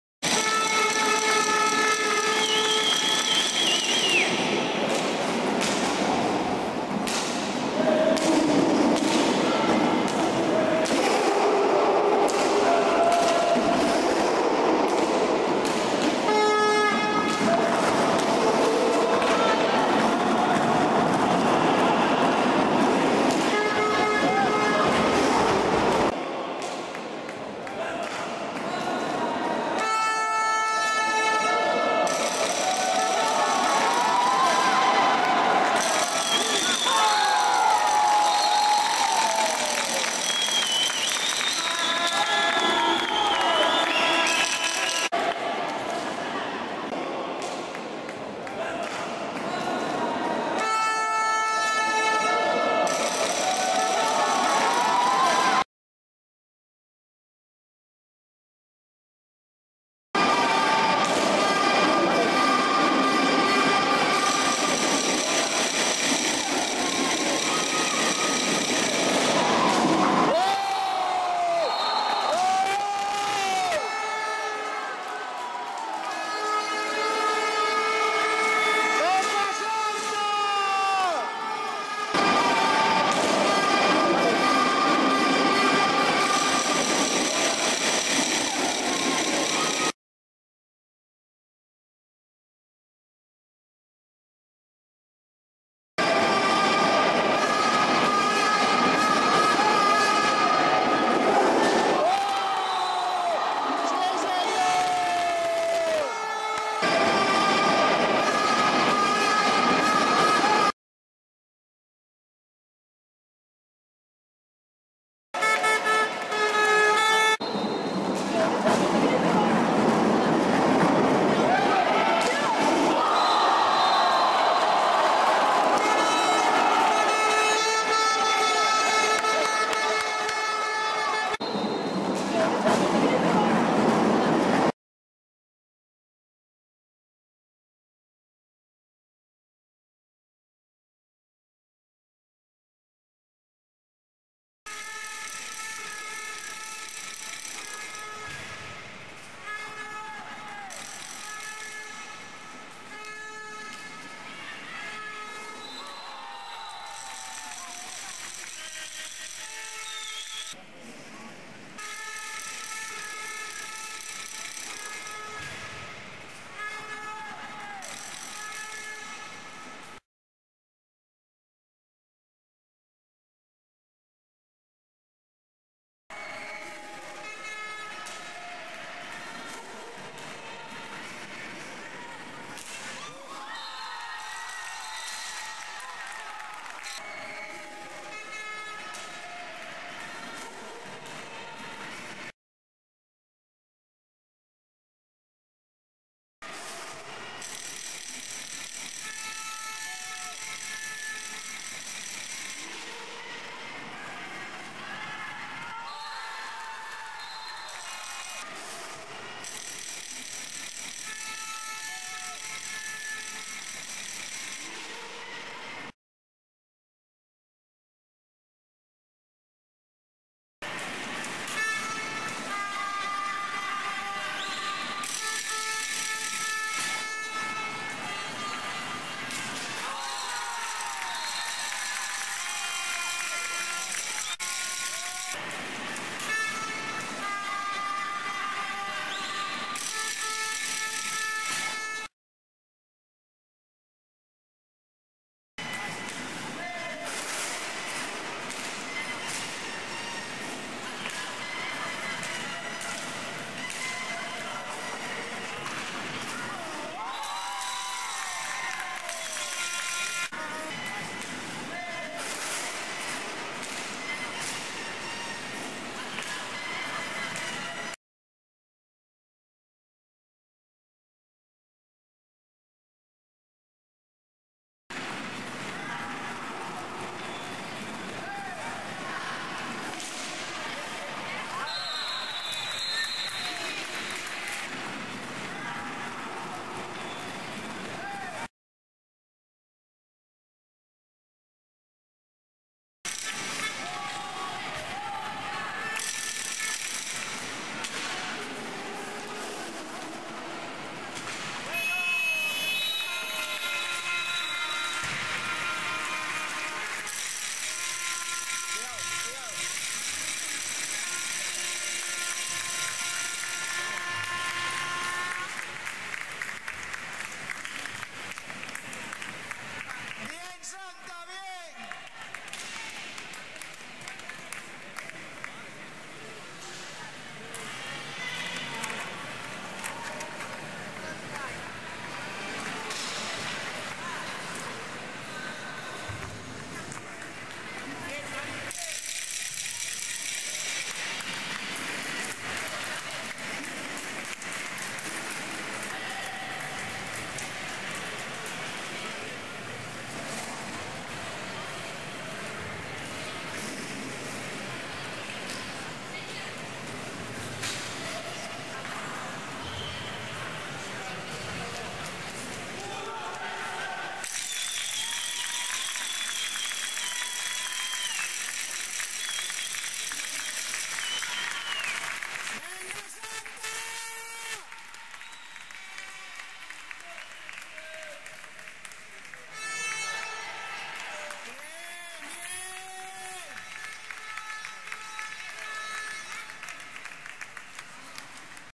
HockeyGlobal.net__Goles.Sta.María del Mar-CP La Corredoria.Sector Norte Infantil. A Coruña 2011.
Video con los goles del partido de Sector norte Infantil , jugado en A Coruña. Sta.María del Mar-CP La Corredoria.